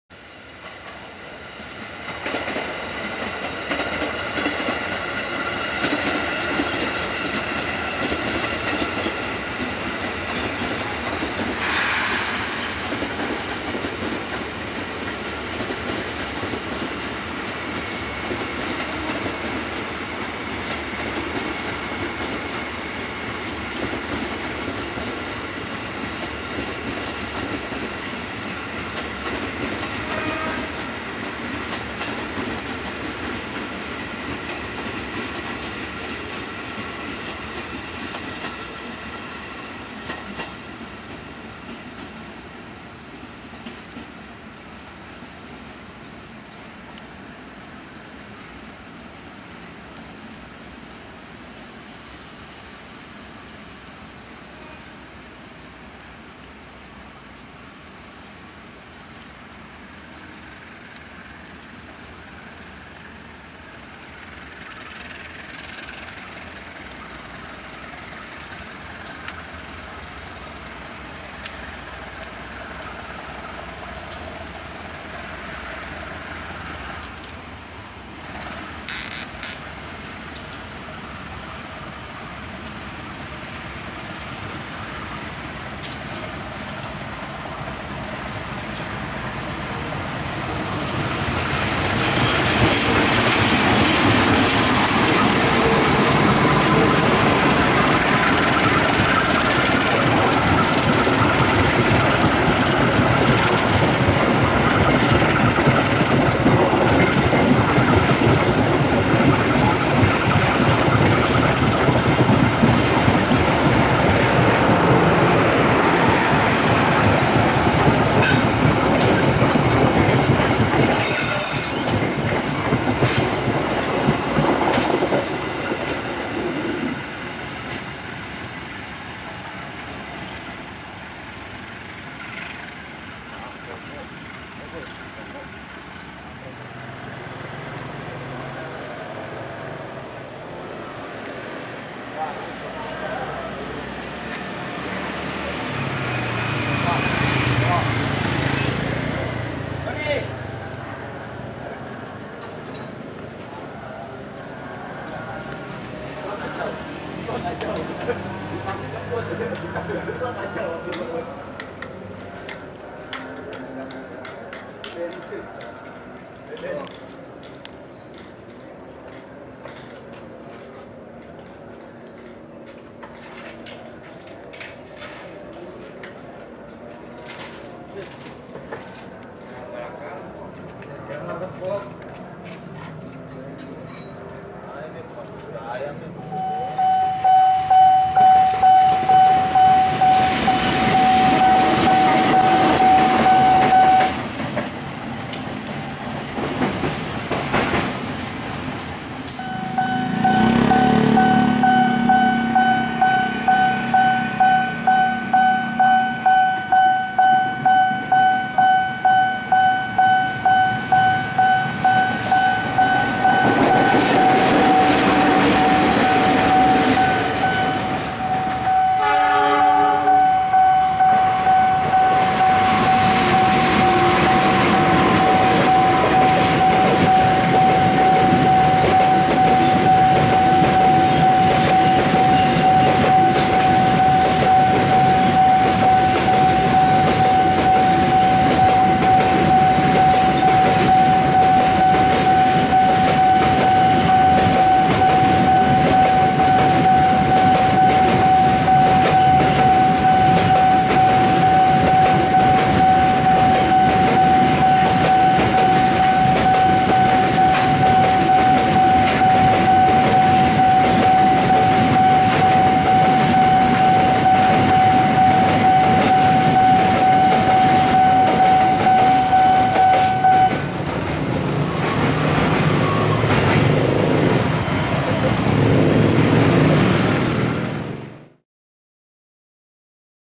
Kereta Malam & Kucing Malam
Matraman, 11-12 Desember 2021

Kereta Malam di Matraman Jakarta Timur

Editings : Wondershare Filmora9 / PhotoDirector / Snapseed
Sound : -